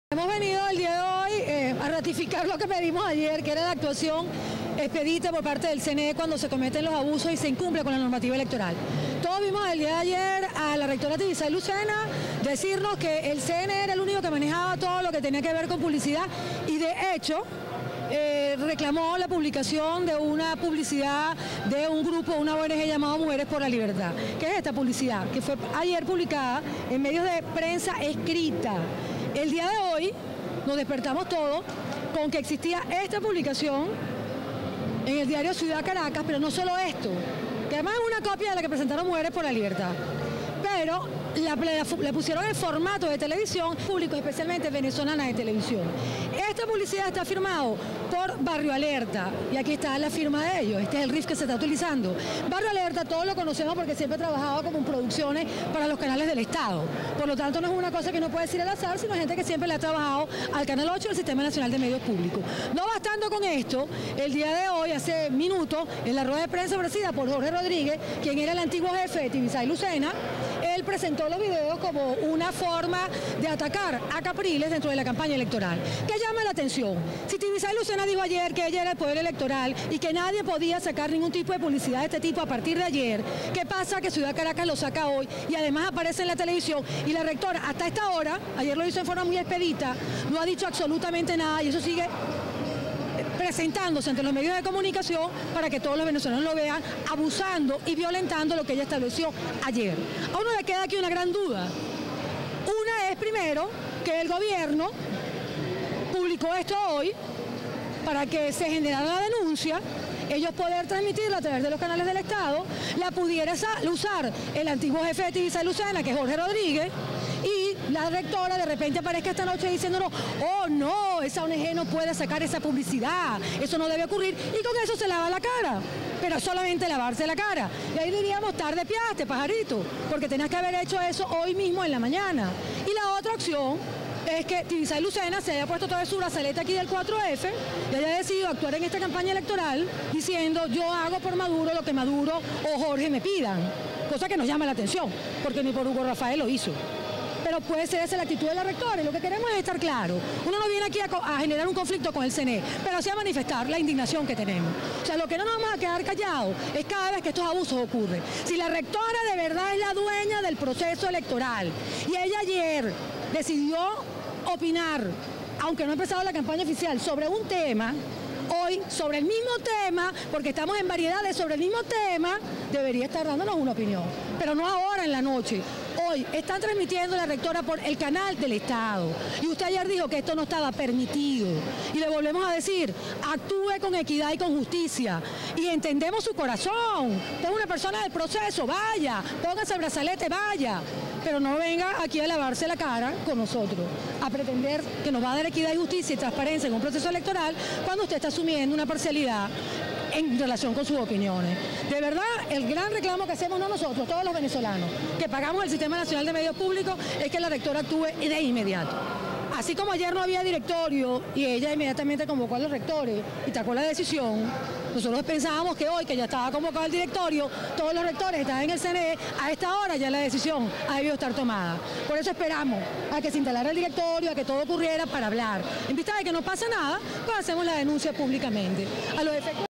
Comando Simón Bolívar denuncia publicación de propaganda oficial prohibida por el CNE
La representante del Comando Simón Bolívar, Liliana Hernández, indicó que la propaganda a favor del candidato de gobierno, Nicolás Maduro, es responsabilidad de Barrio Alerta, ha salido por el canal del Estado y fue mostrada durante la rueda de prensa del Comando Hugo Chávez realizada por Jorge Rodríguez, como una "forma de atacar a (Henrique) Capriles"